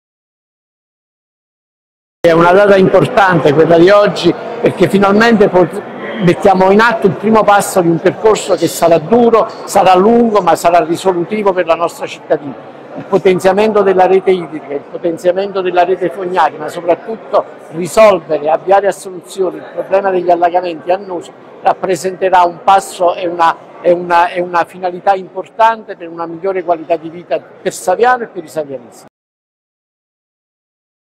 Saviano (NA) - GORI AL LAVORO IN VIA PONTE (19.06.24)
- Saviano (NA) - COMUNICATO STAMPA - GORI AL LAVORO IN VIA PONTE DI SIRICO PER LA REALIZZAZIONE DELLA RETE FOGNARIA . L’OPERA CONSENTIRÀ(19.06.24)